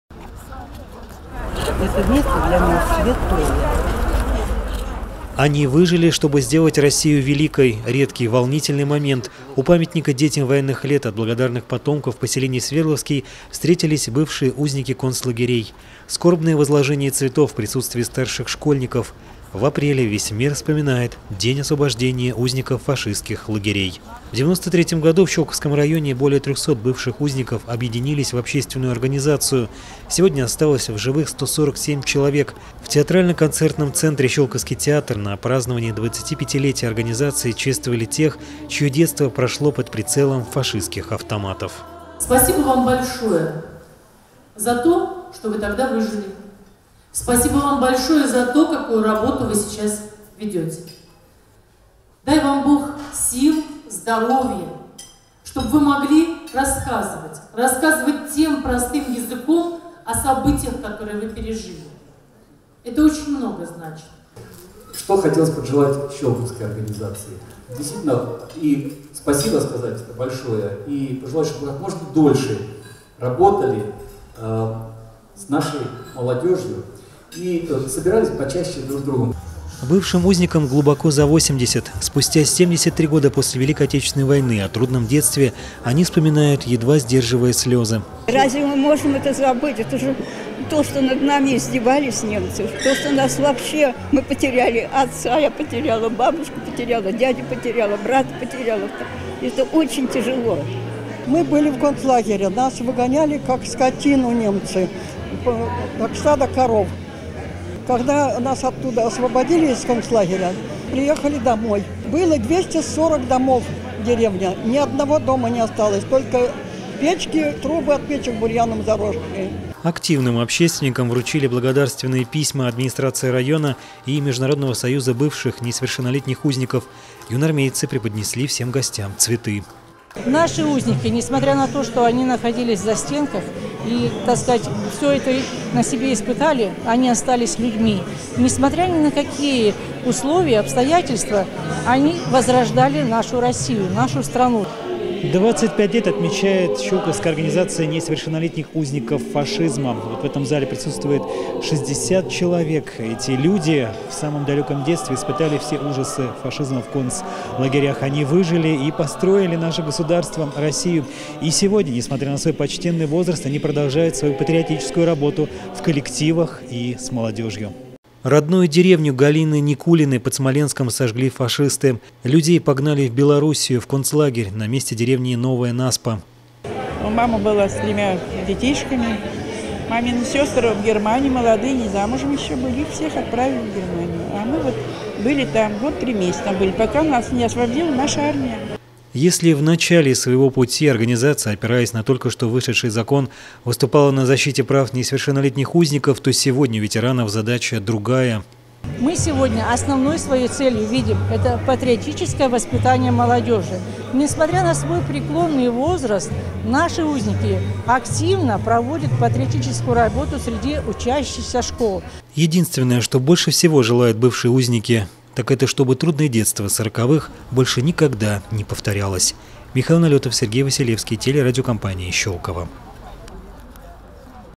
25-летие отметила Щёлковская организация бывших несовершеннолетних узников фашизма
В театрально-концертном центре «Щёлковский театр» состоялось торжественное мероприятие, посвященное 25-летию образования Щёлковской районной организации бывших несовершеннолетних узников фашизма.